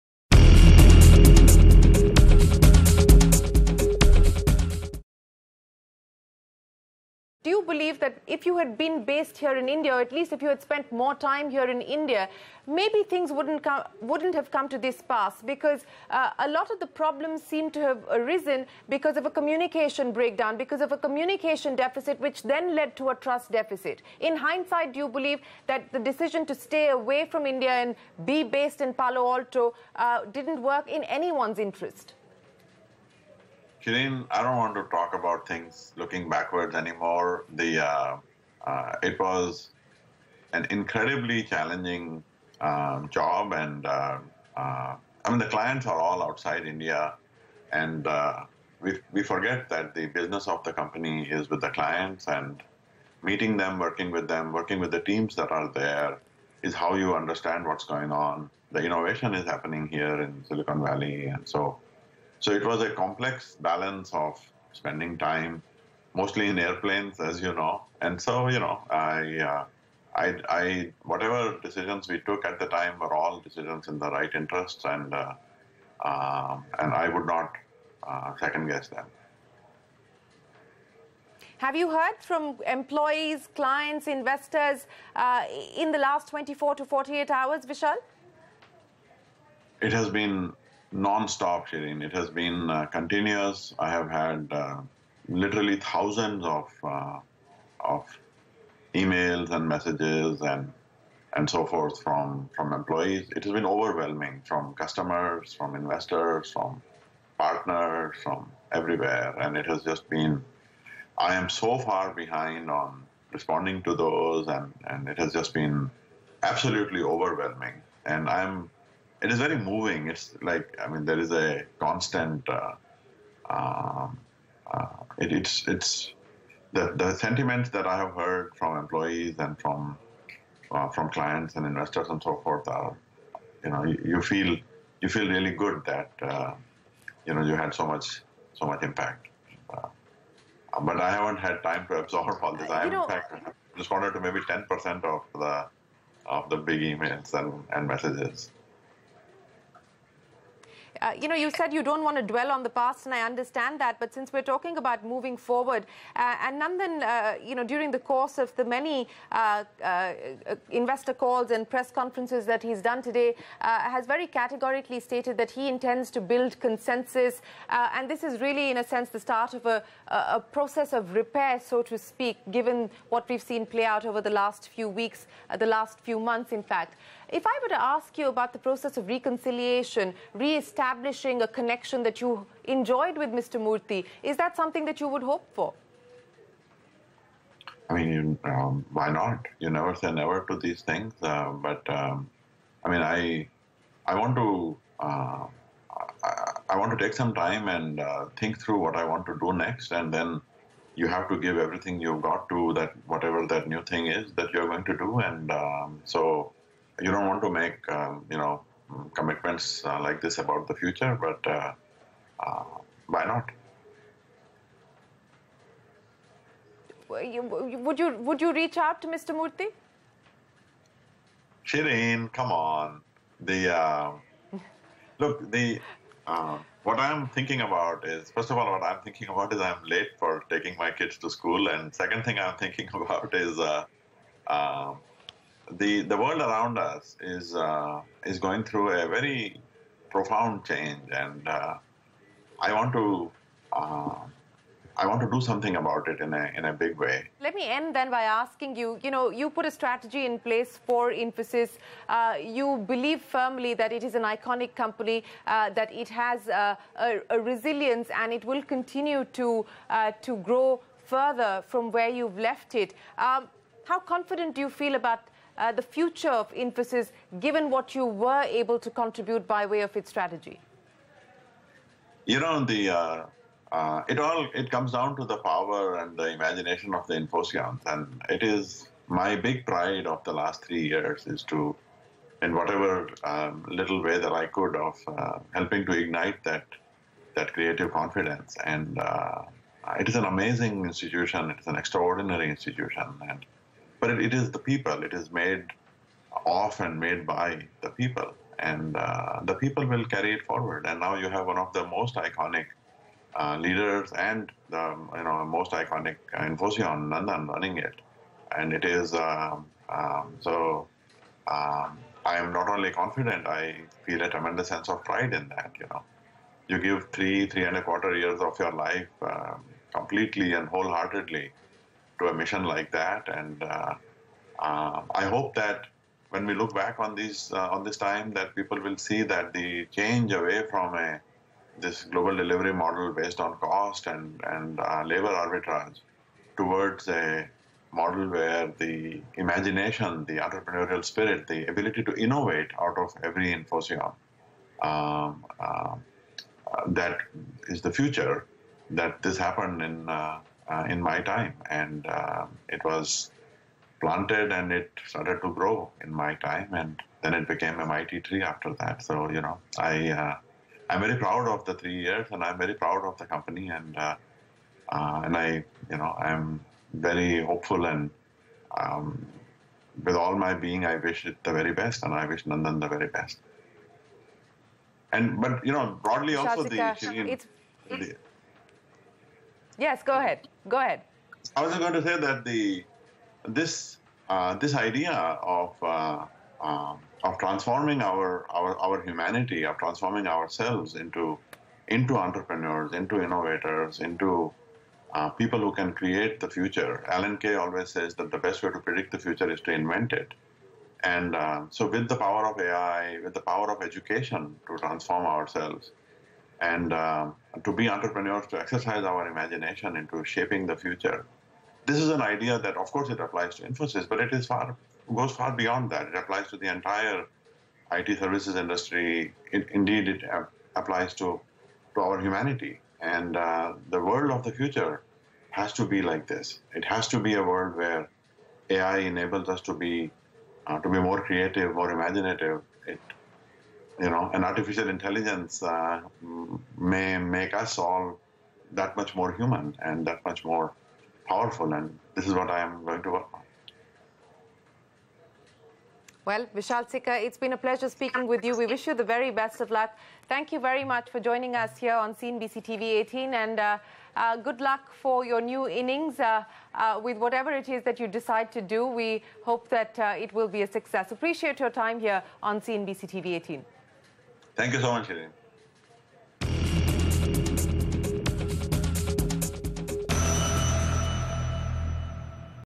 VISHAL SIKKA EXCLUSIVE INTERVIEW - 2
Part 2: CNBC-TV18's Shereen Bhan caught up with Vishal Sikka, his first interview after resigning from Infosys